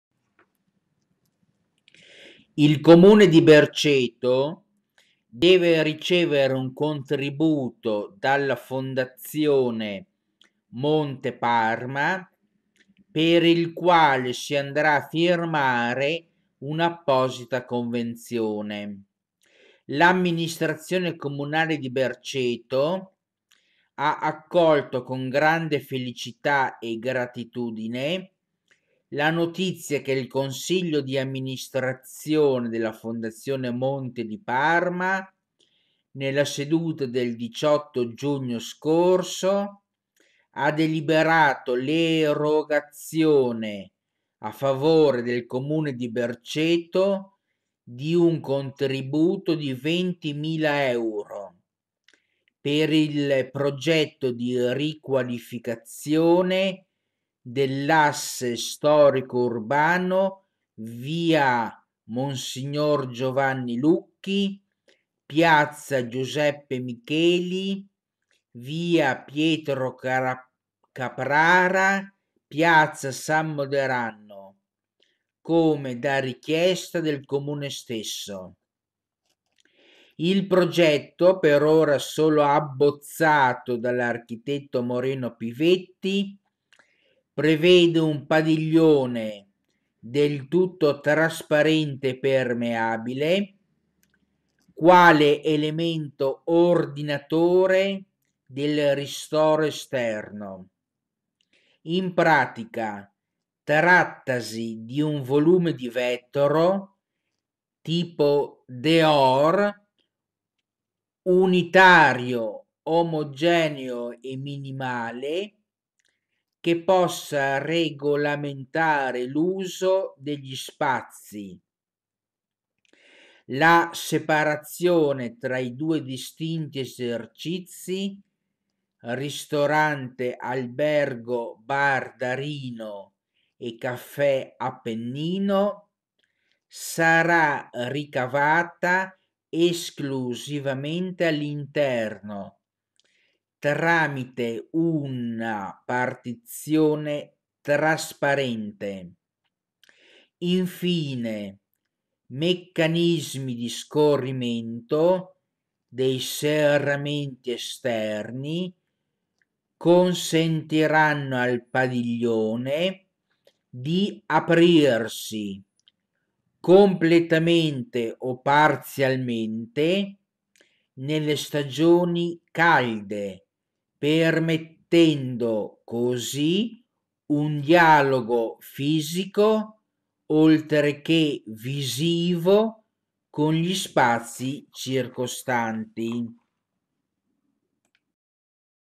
BERCETO - UN UNICO "DEHOR" SIA PER IL CAFFE' "DA RINO" SIA PER IL CAFFE' "APPENNINO"
SIA PER RIORDINARE GLI SPAZI ESTERNI DI PIAZZA GIUSEPPE MICHELI, SIA PER UNA FUNZIONALITA' MAGGIORE E PIU' ARCHITETTONICA DEI DUE BAR.